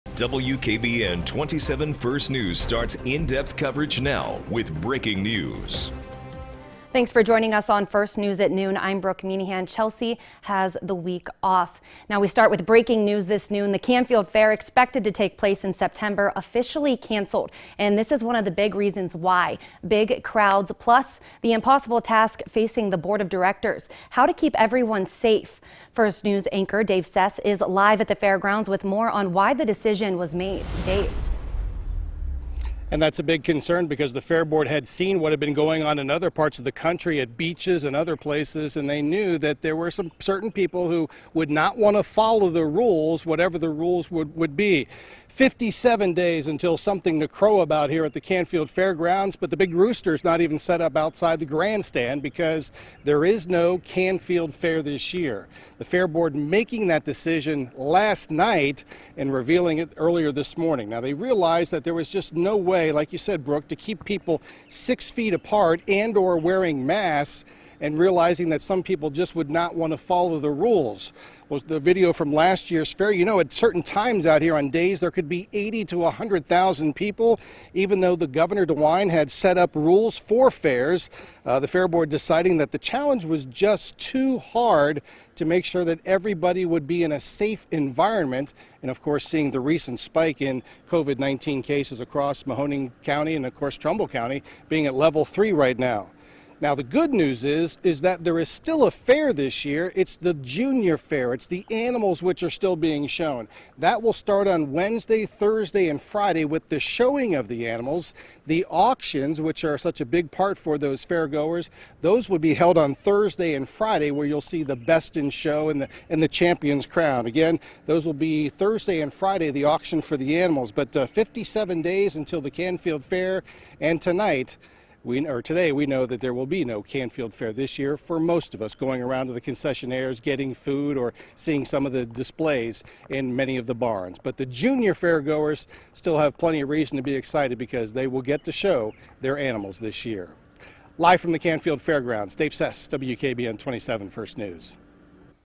Organizers cancel Canfield Fair, citing health concerns
The fair is conducting Junior Fair only